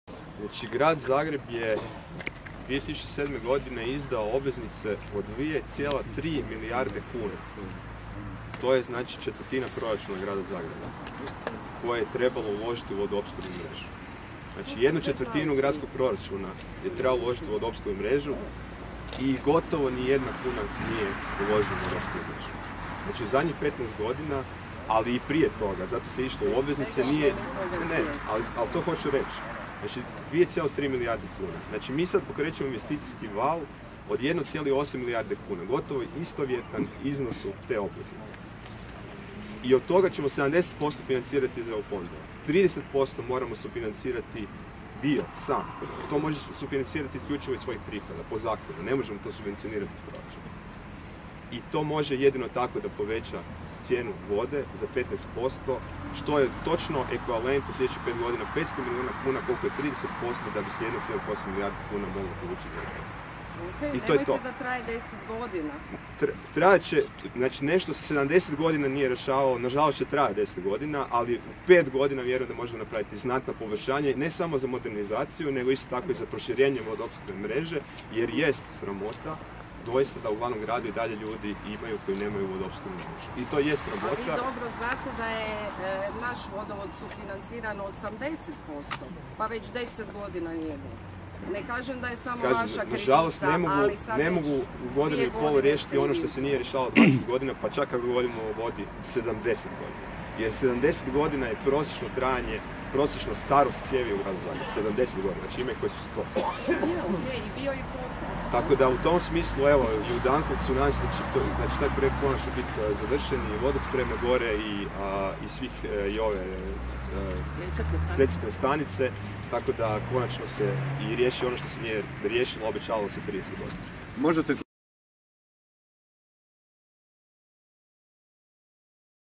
Gradonačelnik Tomašević o poskupljenju vode u Zagrebu
Gradonacelnik Grada Zagreba Tomislav Tomašević, sa zamjenikom Lukom Korlaetom, položio je kamen temeljac za izgradnju novog Društvenog doma Čučerje na Trgu Sv. Marije Čučerske.